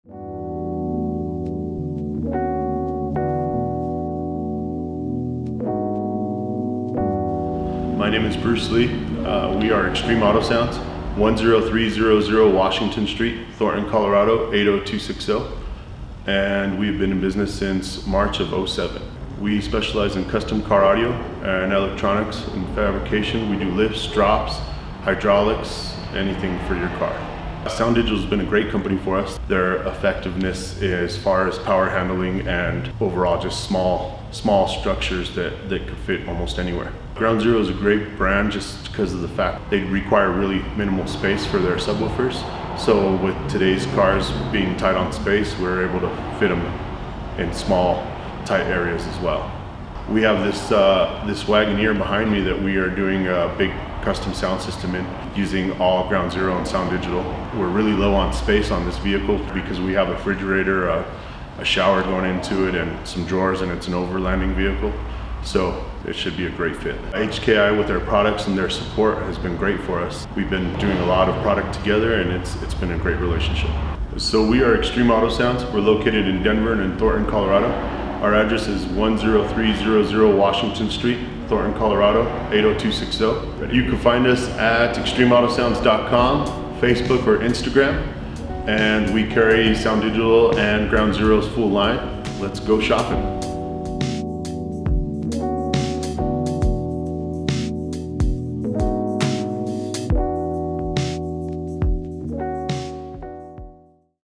HKI Dealer Spotlight - Xtreme Auto Soundz
Check out our HKI Dealer Spotlight, this time with Xtreme Auto Soundz in Denver, CO.

For more information about Xtreme Auto Soundz and the awesome work they do please

For more information about HKI USA and how to become a dealer for Ground Zero and SounDigital please